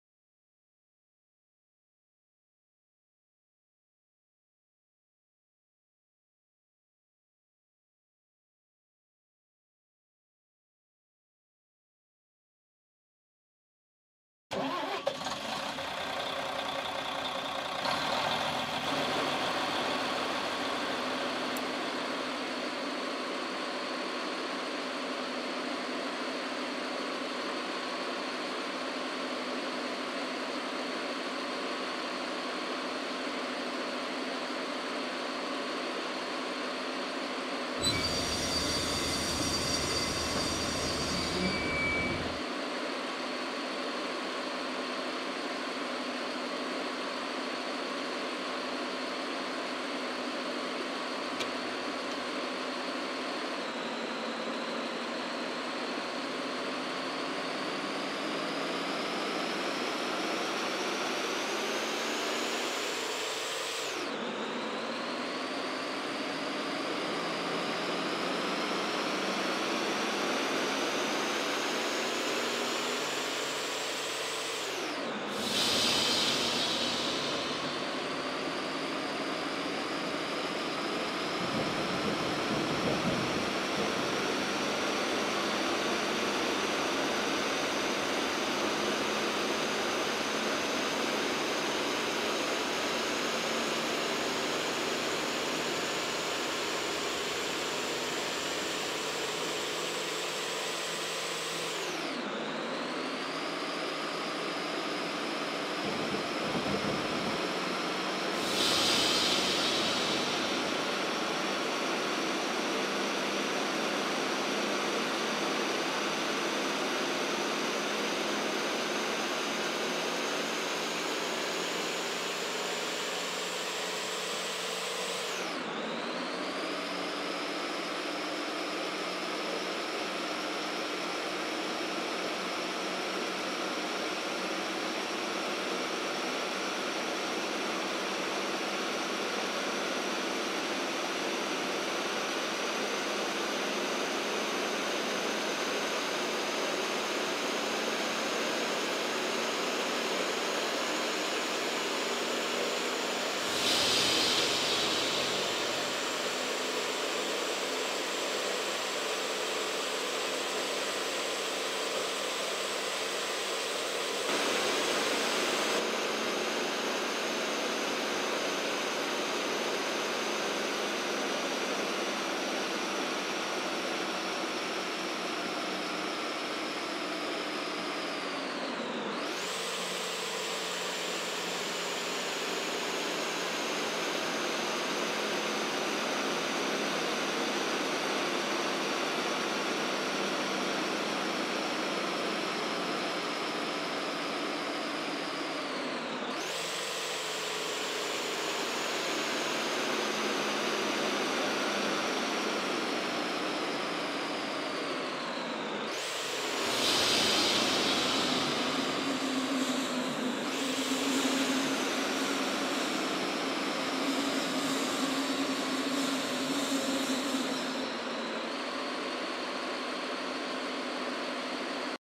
BR_642 Soundtest
Die Br642 gehört zu den sehr schwierig umzusetzenden Fahrzeugen, da das Getriebe einen einzigartigen Sound hat. Jedoch gehört diese Baureihe zu meinen Lieblingsfahrzeugen weshalb ich heute nochmal selbst Hand angelegt habe. Selbstverständlich alles noch WIP.

Heute Testfahrt ab Lth.Wittenberg, fahrtrichtung Leipzig